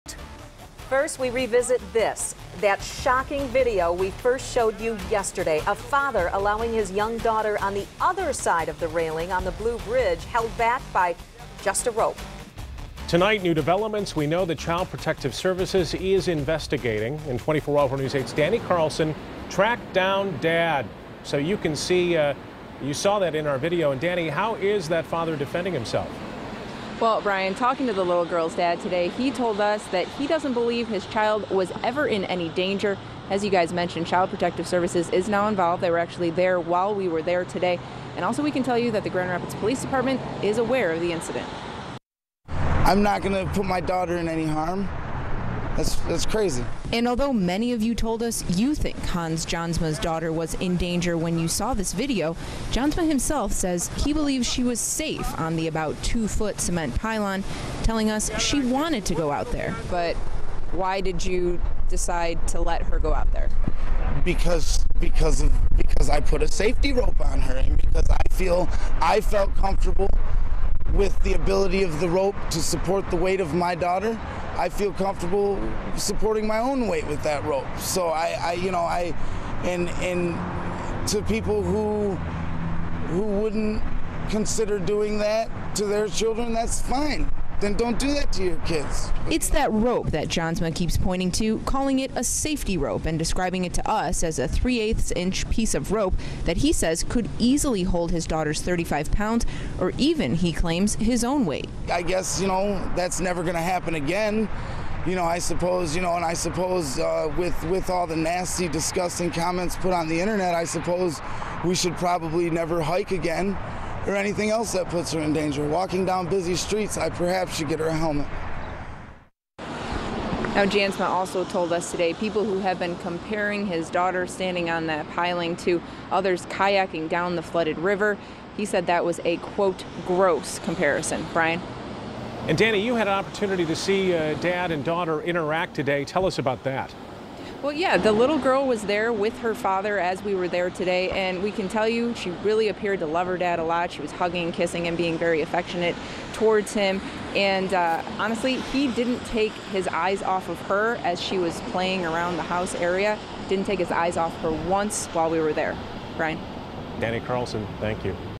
Dad of girl on piling: Never any danger
"I'm not going to put my daughter in any harm," said father Hans Jansma. "That's crazy."